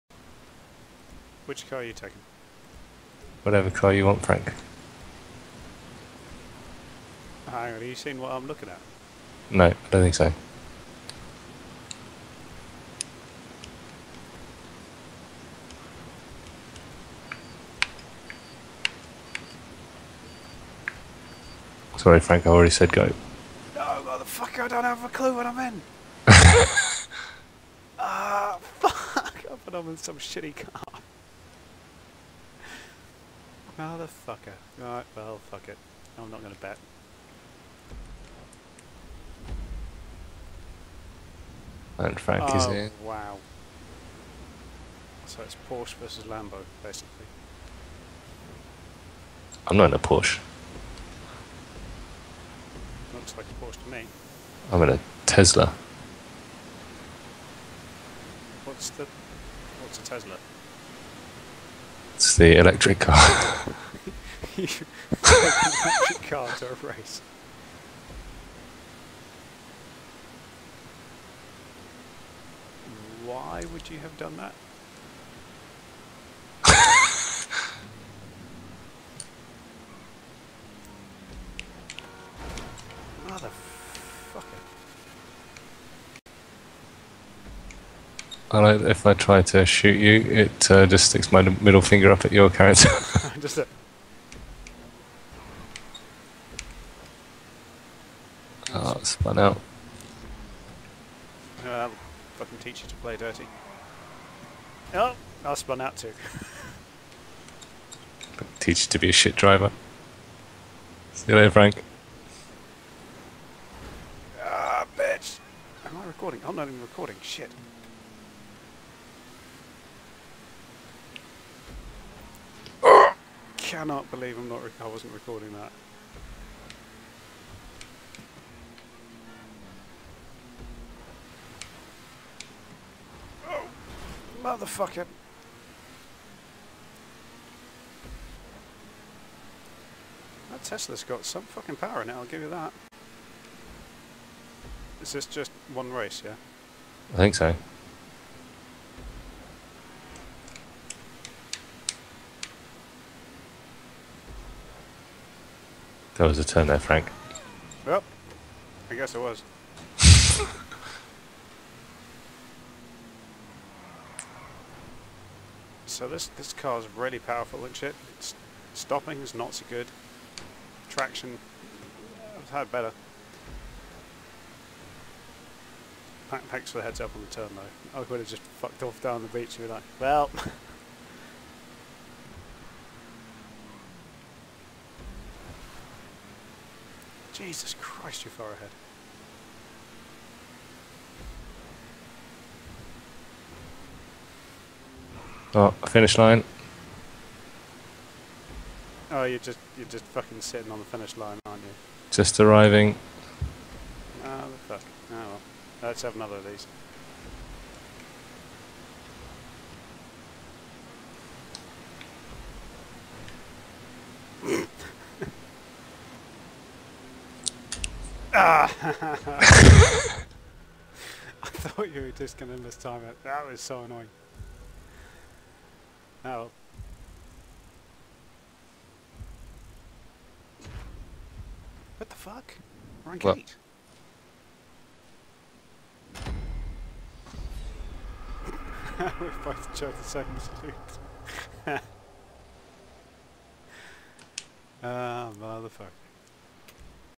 Racing the Tesla
First multiplayer racing attempt... yeah the sound is still bad